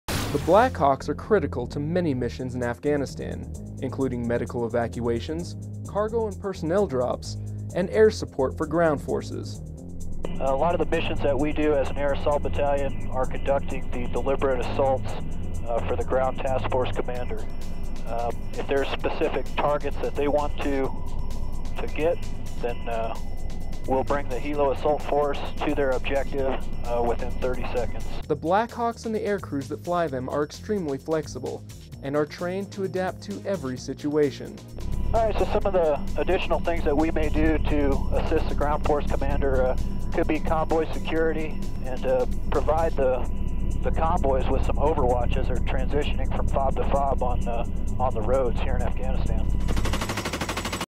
Black Hawks in Afghanistan
Black Hawks are critical for many missions in Afghanistan, including medical evacuations, cargo and personnel drops, and air support for ground forces. Staff Sgt. Joshua Peargin reports from Mazari Sharif, Afghanistan.